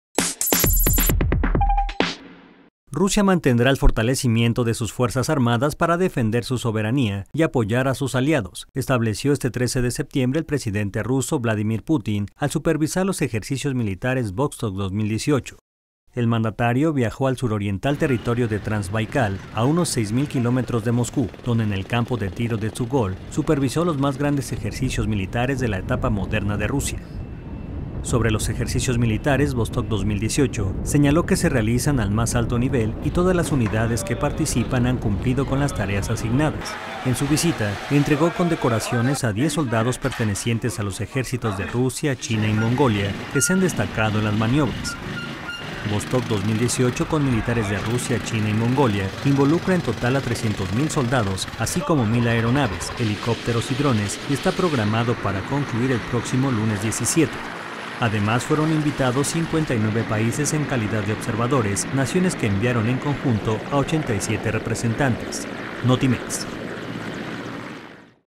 Rusia fortalece ejército para defender soberanía dice Putin
Rusia mantendrá el fortalecimiento de sus fuerzas armadas para defender su soberanía y apoyar a sus aliados, estableció este 13 de septiembre el presidente ruso Vladimir Putin al supervisar los ejercicios militares Vostok 2018.